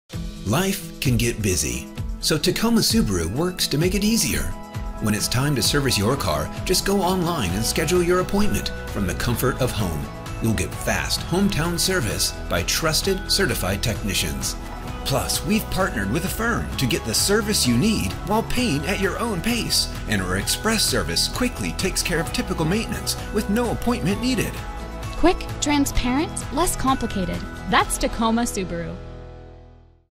Tacoma Subaru Service (Service30)
Life can get busy. So, Tacoma Subaru works to make it easier. You’ll get fast, hometown service... by trusted, certified technicians.

Visit tacomasubaru.com to schedule your next service appointment today!

Address: 3838 S Tacoma Way, Tacoma, WA 98409